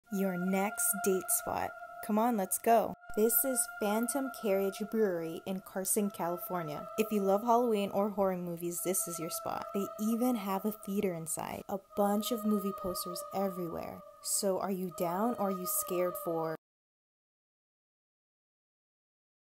Best Halloween/Horror Theme Brewery | Carson California
The best brewery in SoCal! Phantom Carriage Brewery located in Carson, CA is the perfect date night! If you live Halloween or Horror films, this is for you! Will you go?